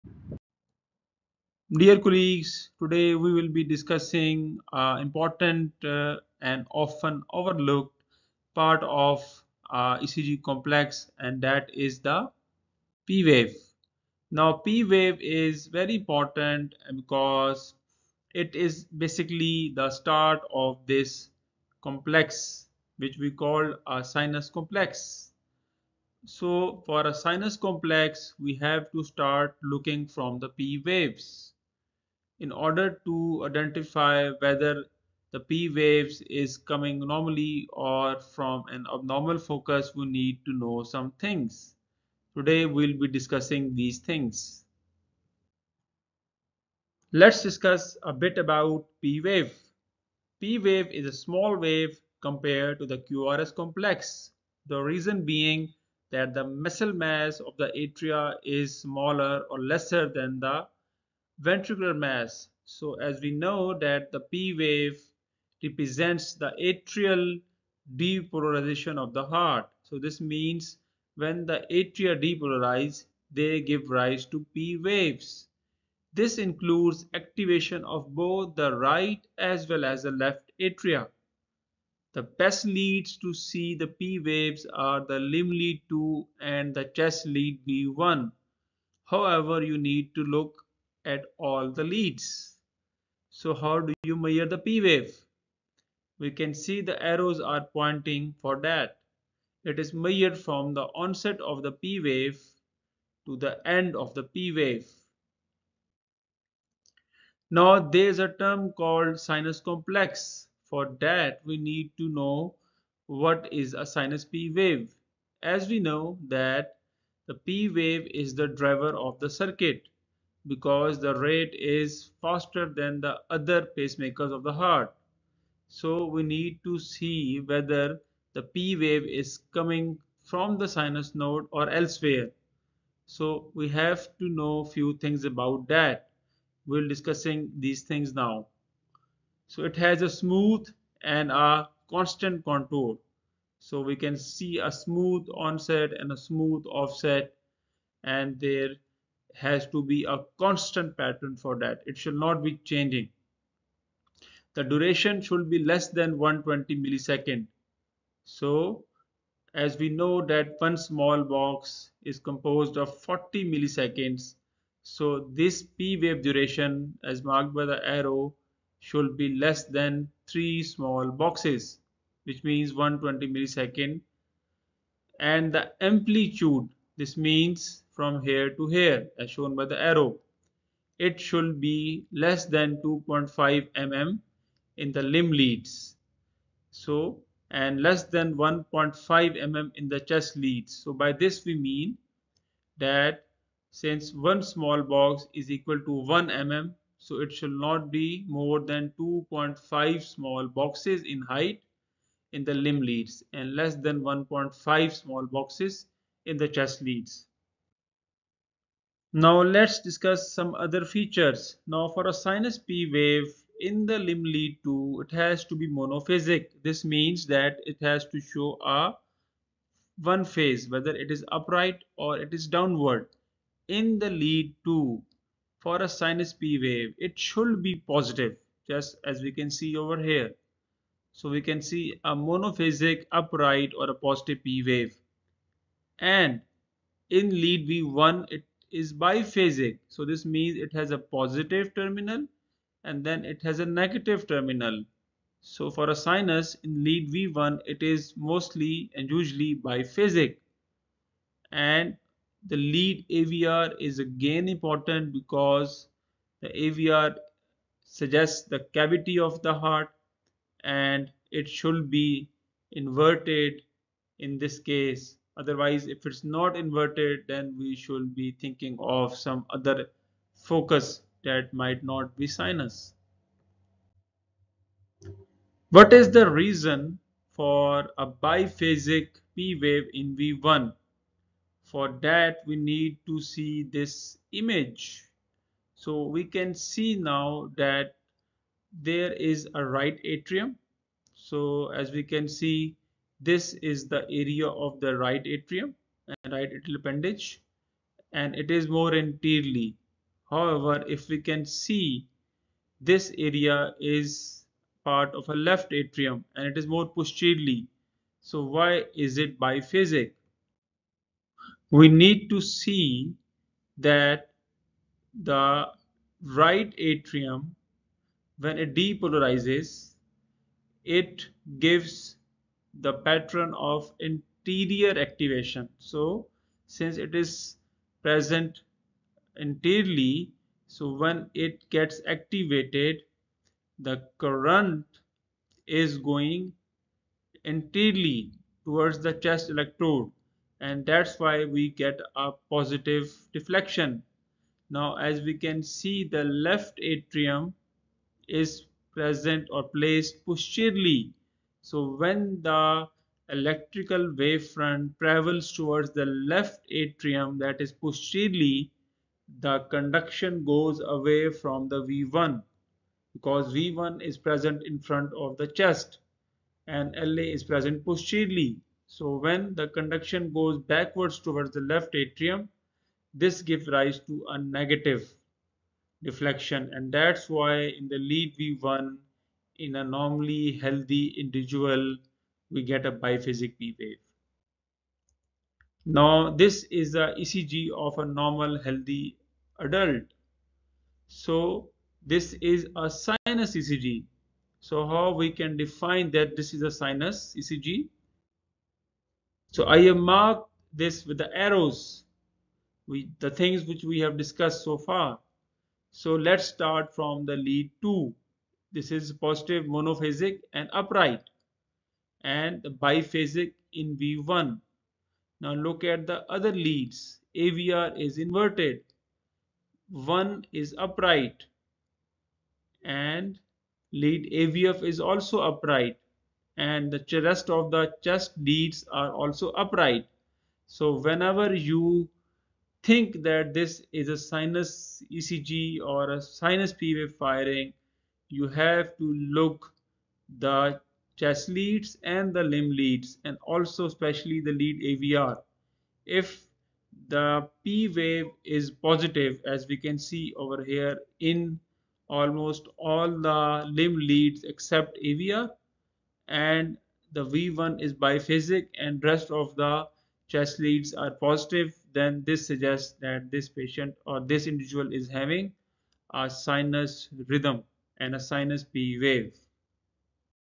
ECG Basics I P waves I Sinus P wave
This educational video for medical students discusses about the P wave and the features of sinus complex in ECG
Following are the features of Sinus P wave

smooth contour
upright in lead 2
biphasic in lead V1
inverted in lead AVR